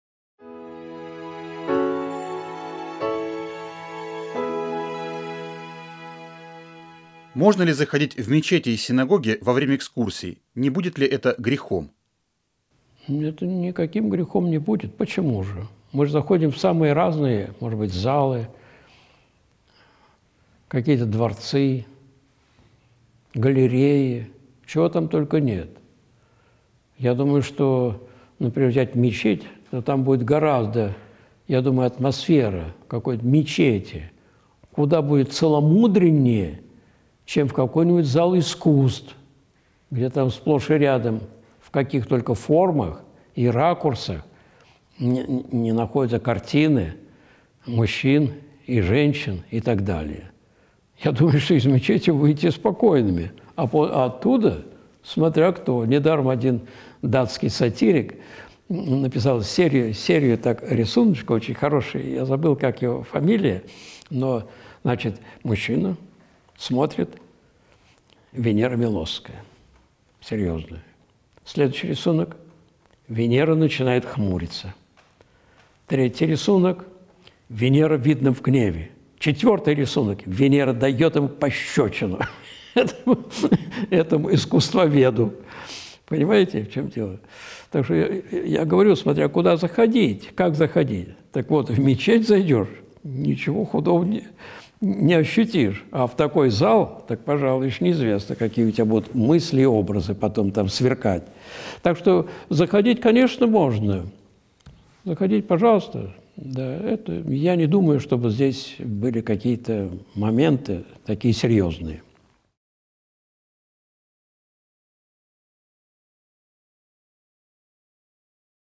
Можно ли заходить в мечети и синагоги во время экскурсий ? Профессор Алексей Ильич Осипов
В сегодняшнем видео вместе с профессором Алексеем Осиповым отвечаем на вопрос: "Можно ли заходить в мечети и синагоги во время экскурсий ?".
Будут затронуты следующие темы:

🔸Нет греха.
🔸Культурные сооружения.
🔸Отсутствие изображений.
🔸Зал искусств.
🔸Мысли и образы.